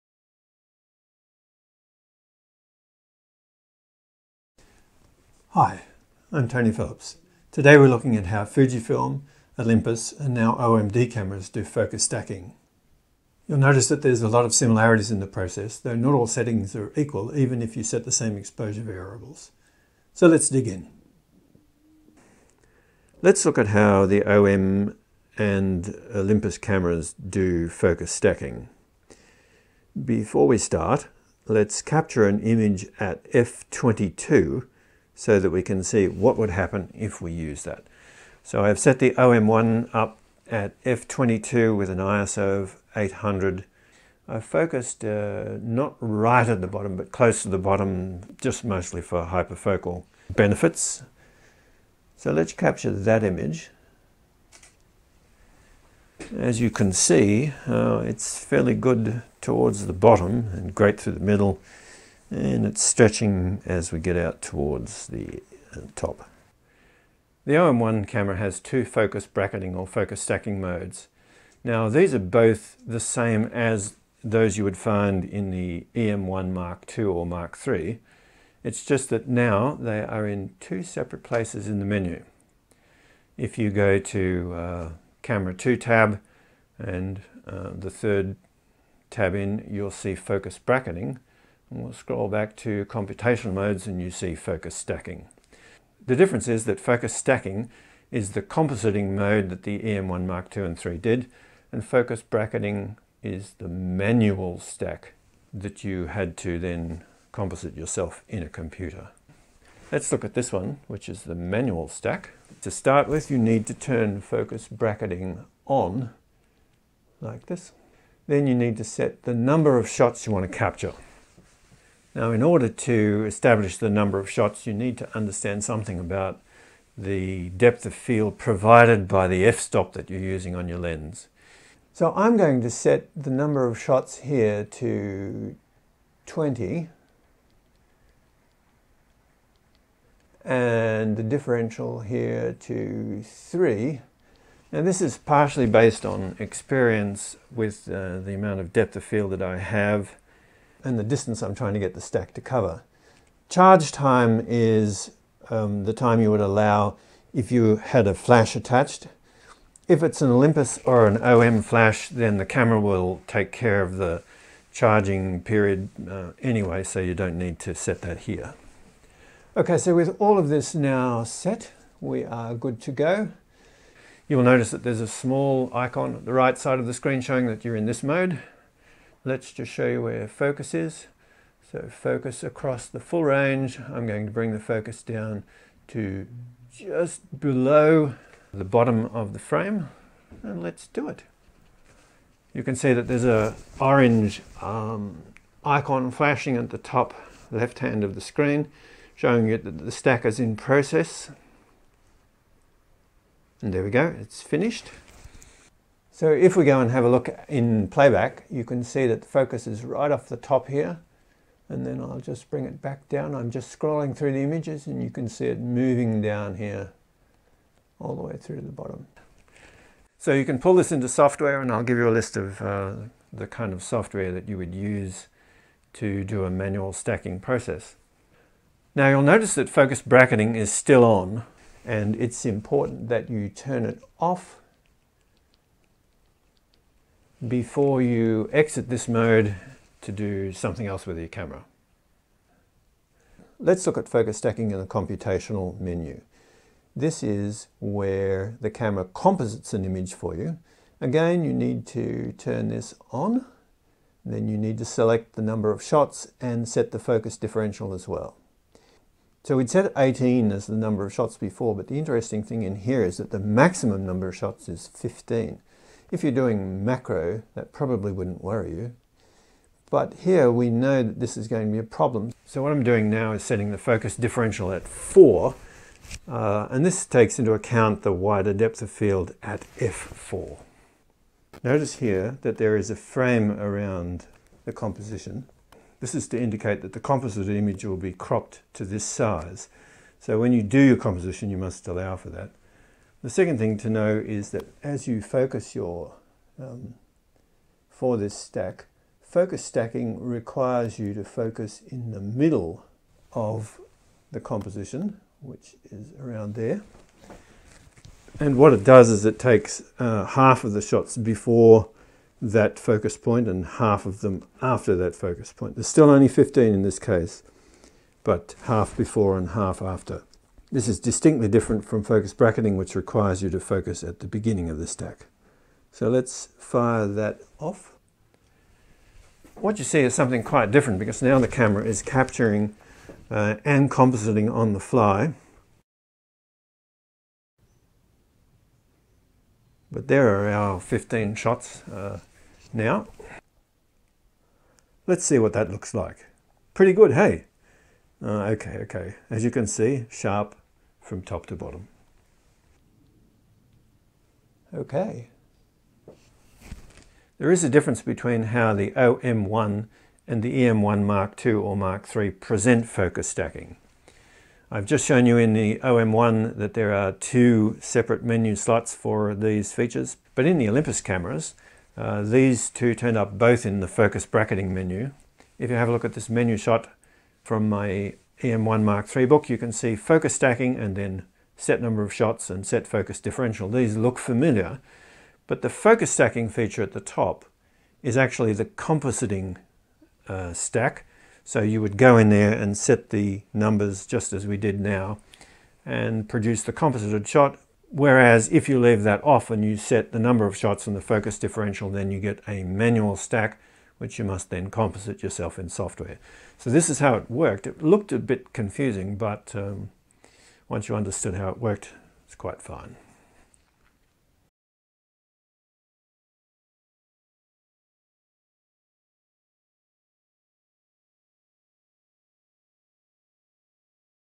Focus Stacking for Olympus cameras
In this video, best-selling e-book author of Olympus cameras Tony Phillips demonstrates the two different ways recent Olympus bodies can perform focus stacking (a technique to ensure razor-sharp details over the entire image).